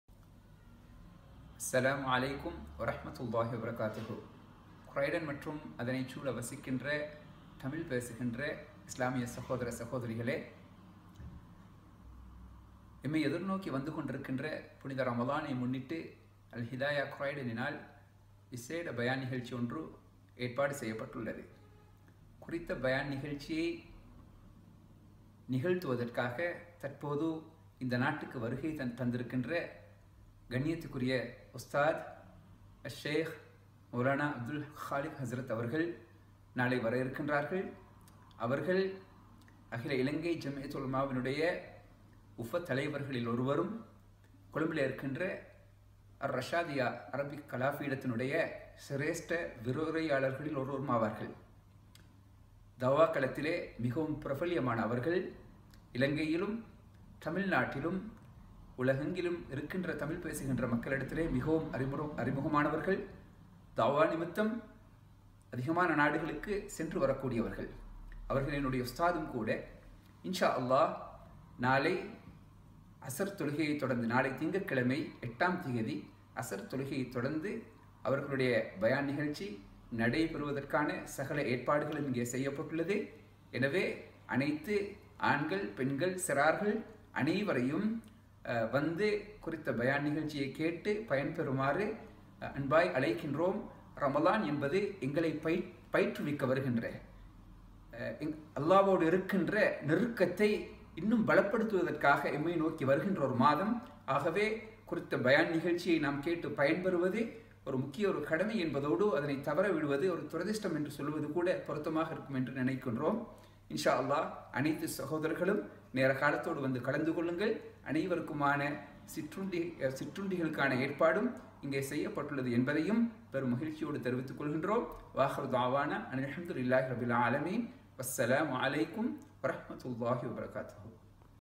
Invite for Welcoming Ramadan Abdul Khaliq
Alhidaya invites you for an inspirational talk on 'welcoming Ramadan 2017'